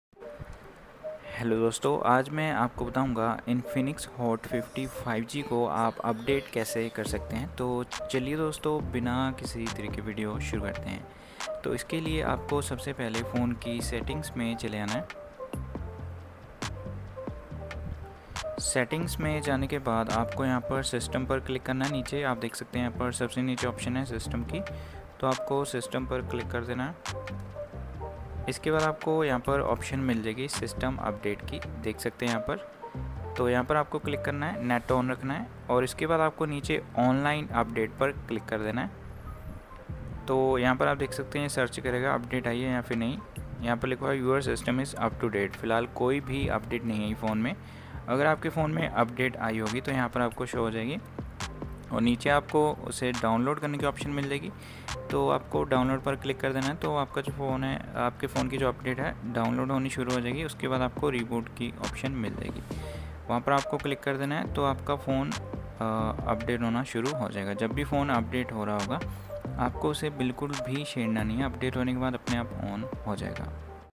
How to update Infinix Hot 50 5G || Infinix Hot 50 5G ko update kaise kare
Hello dosto, aaj maine is video me bataya hai ki Infinix Hot 50 5G ko update kaise kare, dosto Infinix Hot 50 5G ko update karna bahut hi aasan hai. Agar aap bhi janna chahate hain ki Infinix Hot 50 5G ko update kaise kare to is video ko pura dekhiyega, thanks friends.

Hello friends, in this video, I've explained how to update Infinix Hot 50 5G. Updating Infinix Hot 50 5G is very easy. If you want to know how to update Infinix Hot 50 5G, be sure to watch this entire video. Thanks, friends.

How to update Infinix Hot 50 5G
Infinix Hot 50 5G ko update kaise karte hain